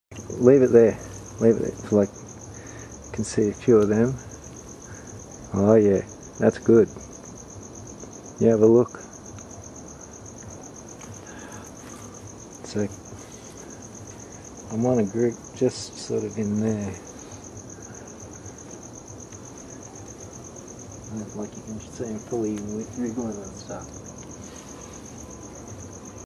Maggots in a worm farm - HD
These are soldier fly larvae that almost completely took over my worm farm recently. They are voracious consumers of compostable materials and that's a good thing in itself if the intended use of the castings is primarily as a soil conditioner (rich in polysaccharide gels) or biological activitator/inoculant. As a fertilizer however, if the larvae are allowed to pupate, hatch and then fly away the castings may end up containing substantially less phosphorous and nitrogen.  Also, the compost worms might not be able to compete for space with the fly larvae, rendering the farm worm-less as is what nearly happened here. I now use fly screen to stop the soldier flies from laying eggs in the worm farm.